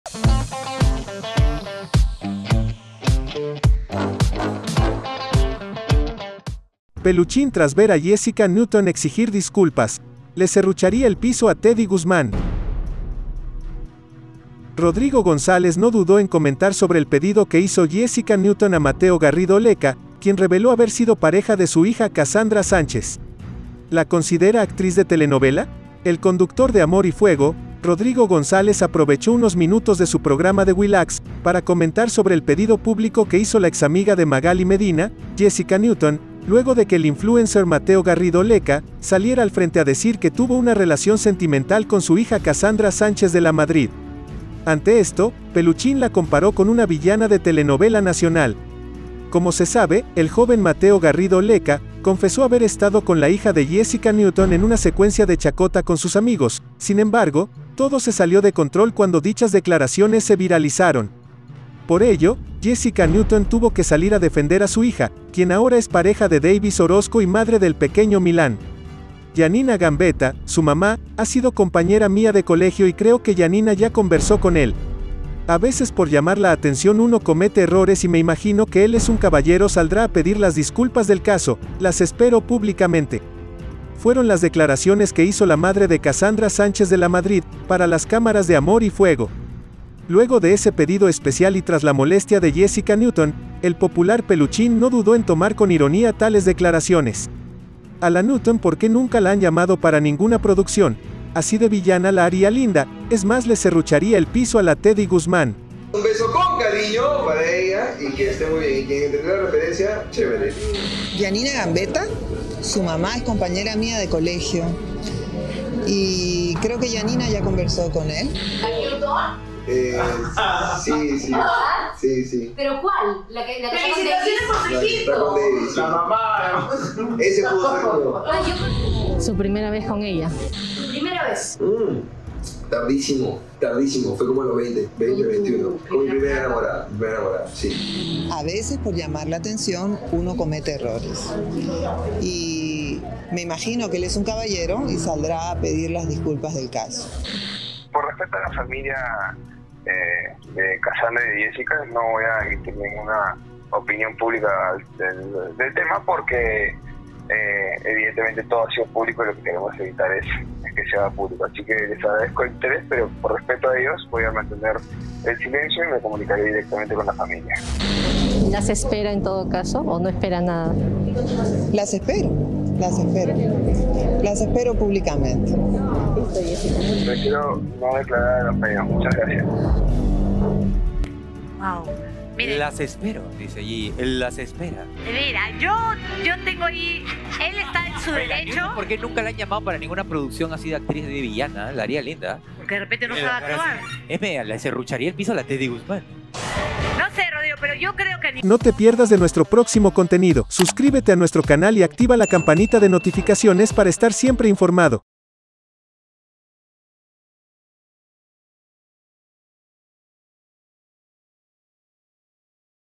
MATEO GARRIDO LECCA no QUIERE pedir DISCULPAS PUBLICAS a CASSANDRA SANCHEZ
Rodrigo González no dudó en comentar sobre el pedido que hizo Jessica Newton a Mateo Garrido Lecca, quien reveló haber sido pareja de su hija Cassandra Sánchez.